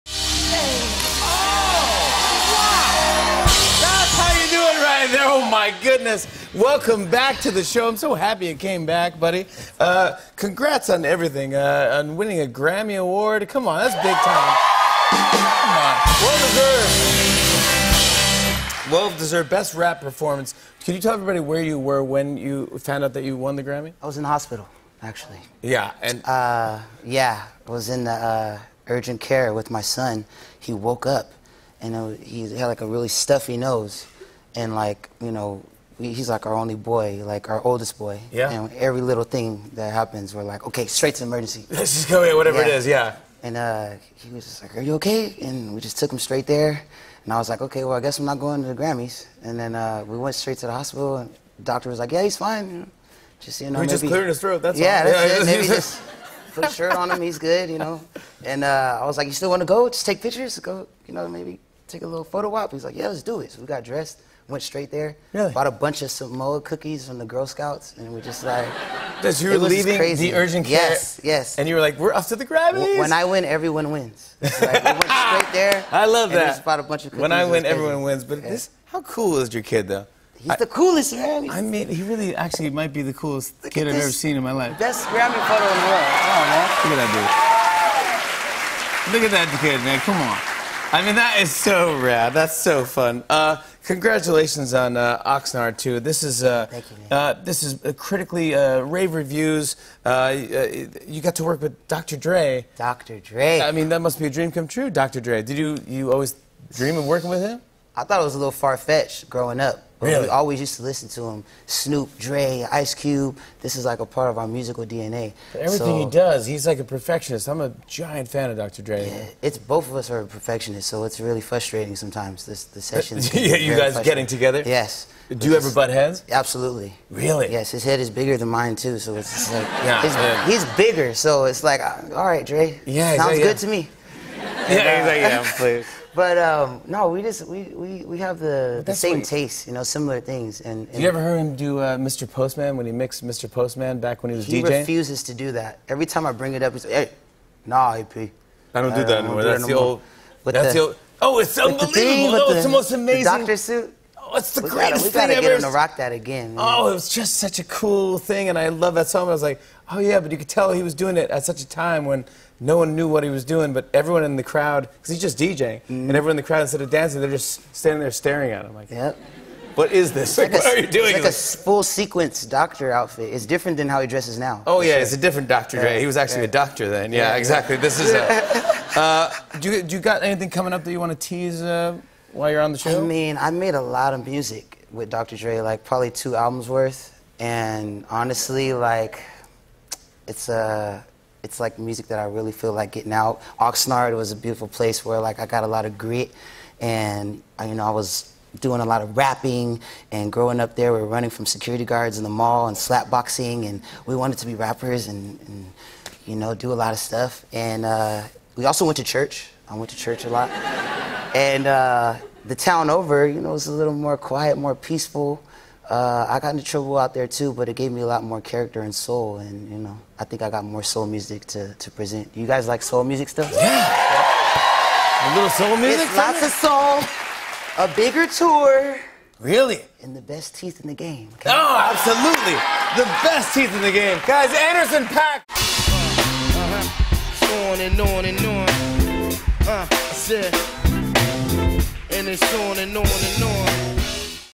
Anderson .Paak Butt Heads with Dr. Dre in the Studio
Anderson .Paak talks about being in the urgent care with his son when he learned he won his first Grammy and his perfectionist-driven album recording sessions with Dr. Dre that resulted in two albums worth of material.